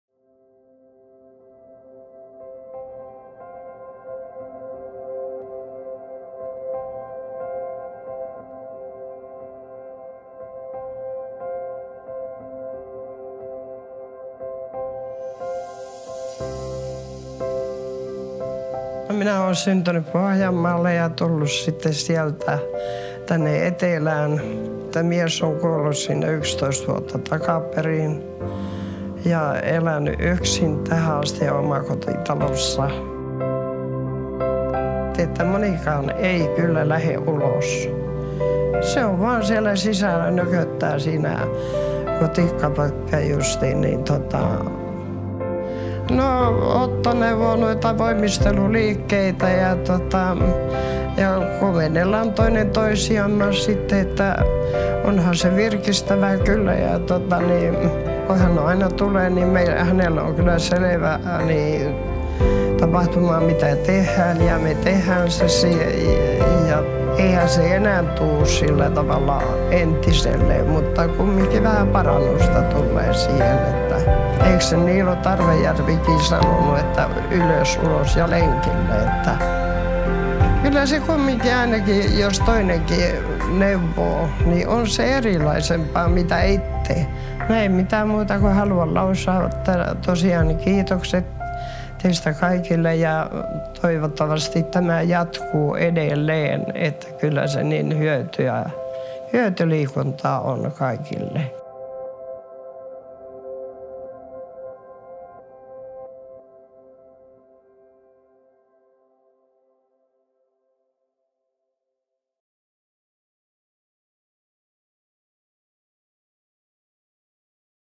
Wirtaa Vihtiläisille Vanhuksille
Vihdissä kolmas sektori järjestää mittavaa senioriliikuttamista. Hankkeen takana ovat Tuovi Siiri Hännisen säätiö ja Nummelan Palloseura tukenaan Karviainen ja Vihdin kunta
Videon tuotannosta ja toteutuksesta vastasi Joenrinne Films Eero Joenrinne ja Petri Munck